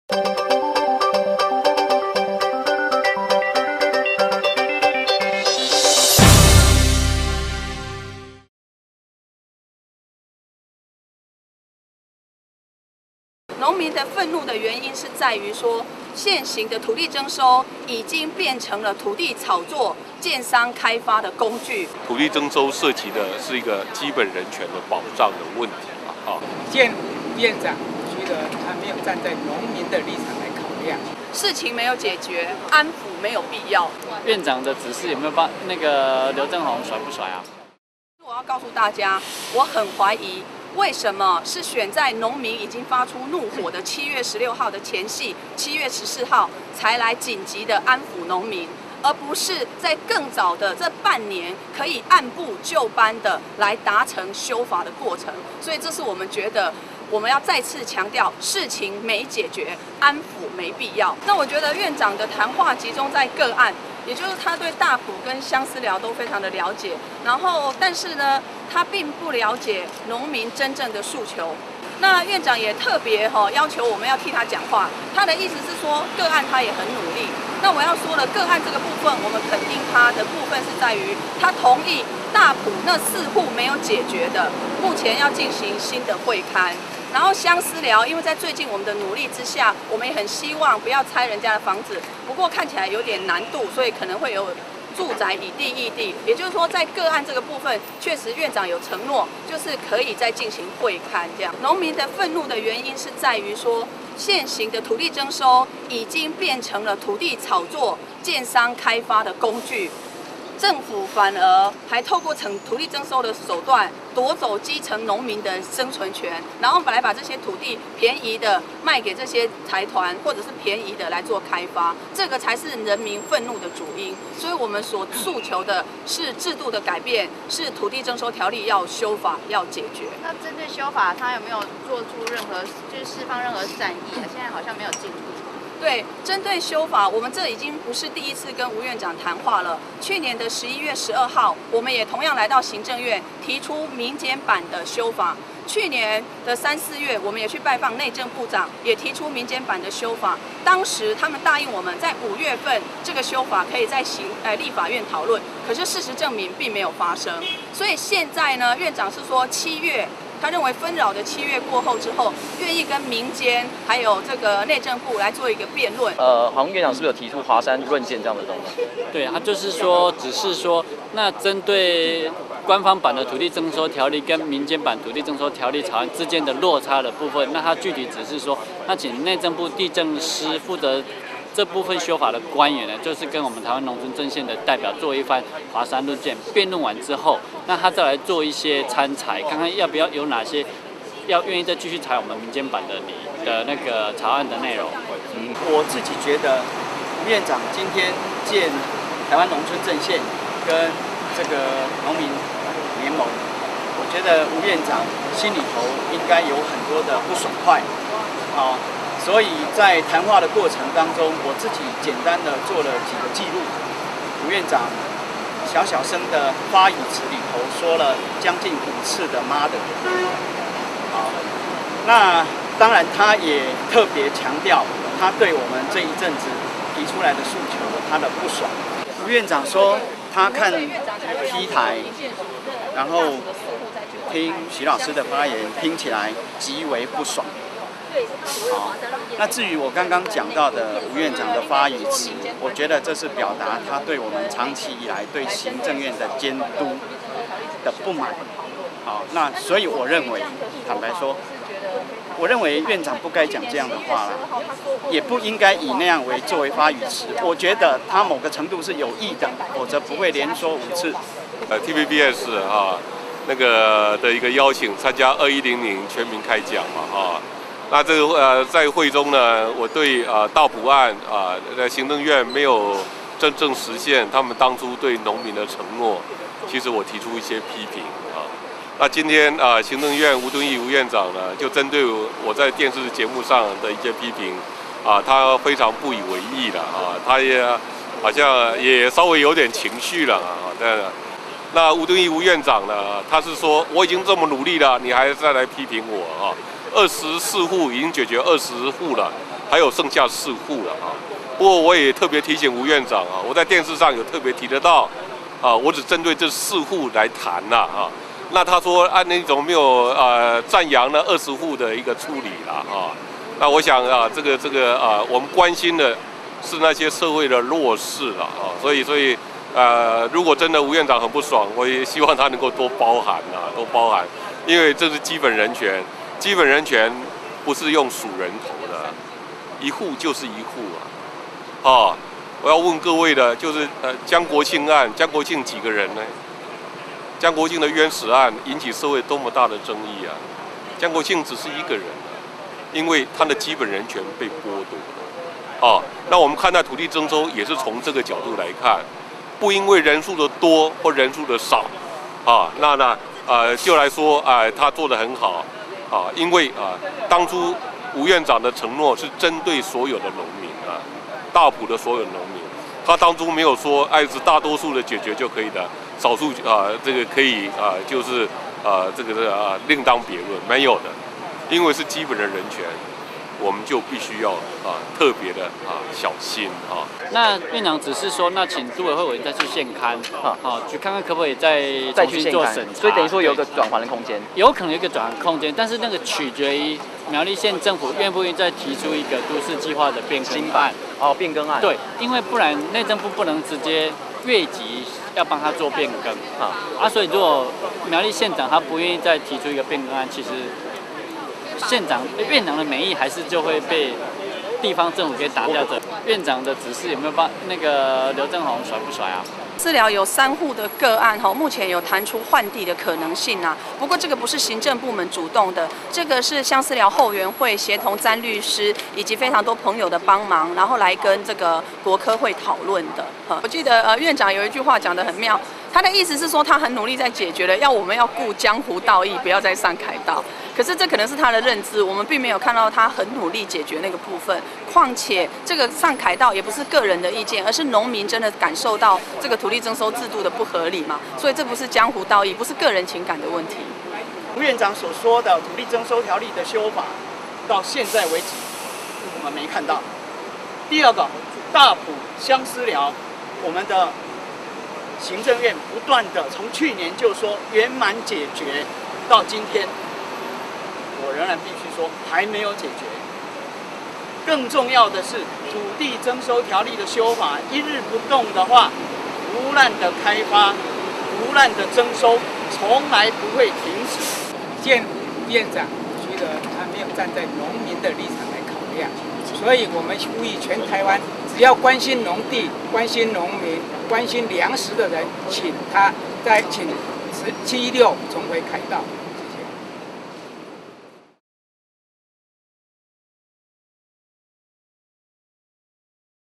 2011.07.13 716農民重返凱道前夕 吳揆臨時約見民間代表
716台灣農民重返凱道日期將即，全台農民聯合自救會「捍衛農鄉聯盟」與台灣農村陣線臨時接到行政院通知，表示吳揆希望13日約見「堅持土地正義、力抗搶水圈地」716農民重返凱道行動代表。由劉慶昌、徐世榮、詹順貴、廖本全、蔡培慧等五人面見吳揆。向吳揆再度力陳，土徵條例應立即修正，不過，吳院長並未承諾具體修法期程，僅要求內政部於七月過後與民間團體「華山論劍」。針對此點，台灣農村陣線強烈表達不滿，早在去年十一月十二日，農陣與農盟已在行政院當面向吳院長提出民間版修法版本，當時，吳院即要求內政部參酌民間版意見，加速修法，言猶在耳，土徵法修法草案還出不了行政院大門，如今，所謂的「華山論劍」所指為何？令人費解！

周末７１６－７１７農民團體將發動大規模的動員，展現人民的力量，堅持土地正義。